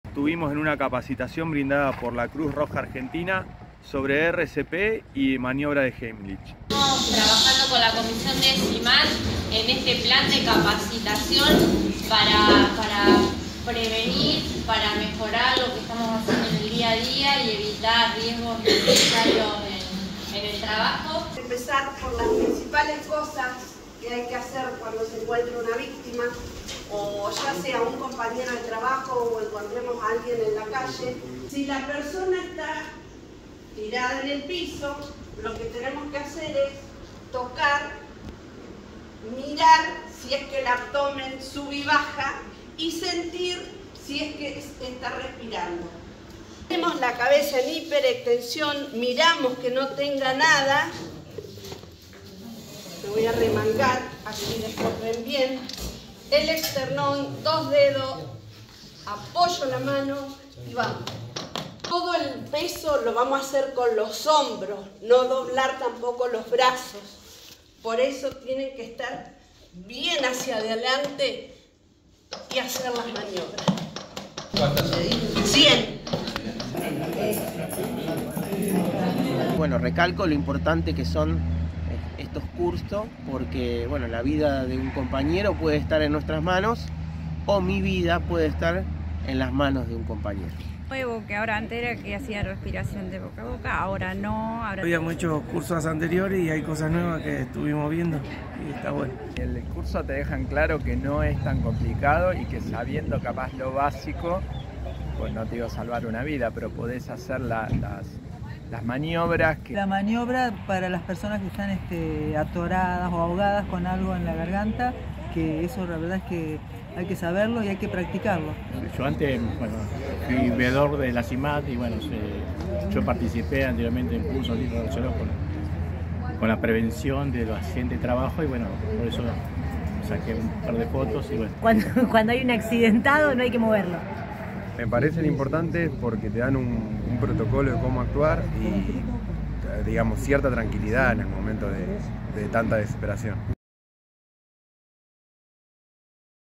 Taller "RCP Reanimación cardiopulmonar" para personal de la EEA San Pedro
Brindado por la Cruz Roja Argentina Filial San Pedro, cuya institución matriz (Cruz Roja Argentina) tiene un convenio con el INTA, se concretó una capacitación centrada en la RCP (Reanimación Cardiopulmonar) y maniobra de Heimlich.
Gran parte del personal estuvo presente, y al final compartieron sus aprendizajes en el video.

La actividad es parte del programa anual de capacitaciones que lleva adelante la Comisión de Condiciones y Medio Ambiente de Trabajo de la Estación Experimental Agropecuaria San Pedro.

Realización: Lorena Peña y Mariana Piola
Junio 2024